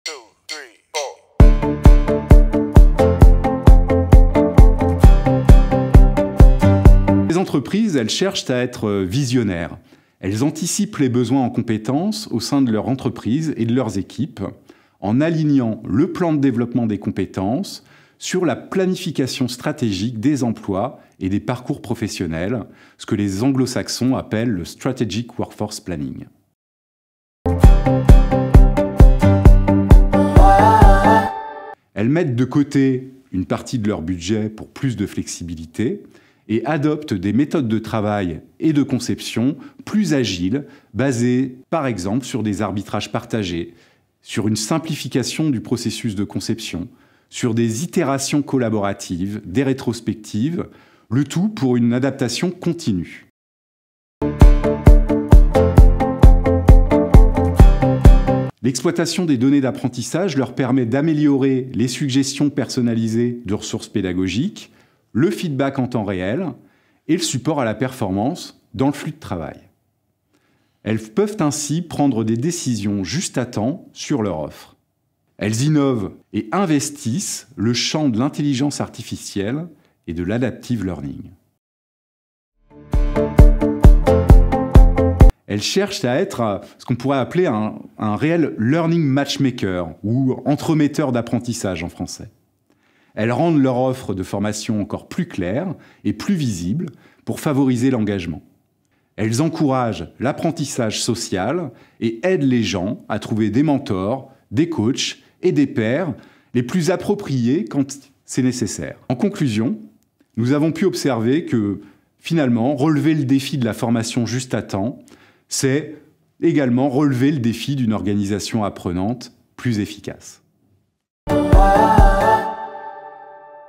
Les secrets de la formation juste-à-temps
La formation juste-à-temps est une stratégie qui permet aux directions Learning & Development de proposer les formations dont les salarié.es ont besoin au moment où ils/elles en ont besoin. Pour relever ce challenge, il faut faire évoluer sa posture et ses pratiques. Grégory Gallic, manager de l'offre Ingénierie pédagogique Cegos vous en dit plus.